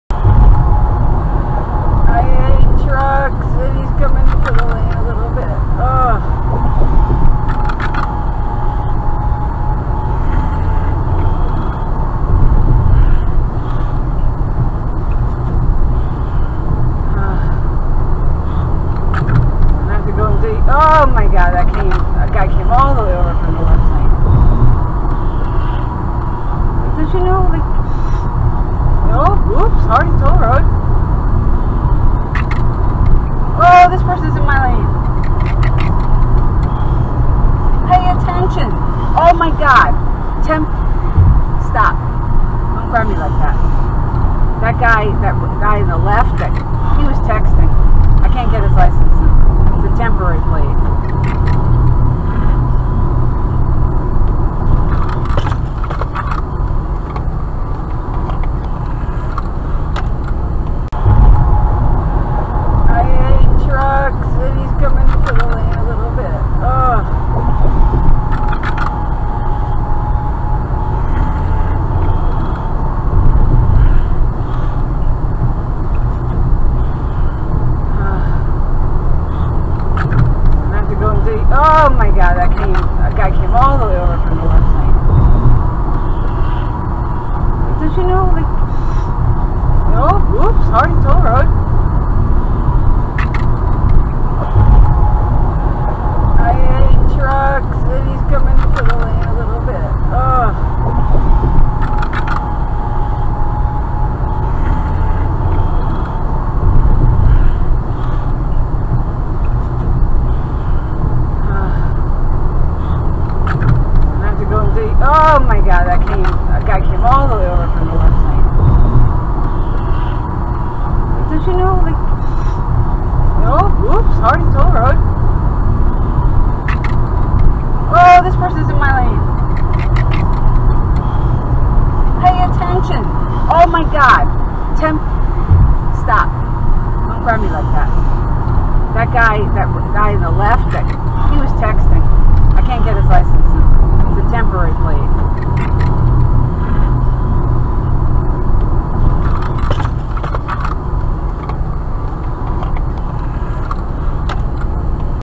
2018 08 28 I45 People Crossing Lanes
First the facing-forward point of view, and then the rear point of view from the rear camera (so you can see what I'm cringing about or yelling about).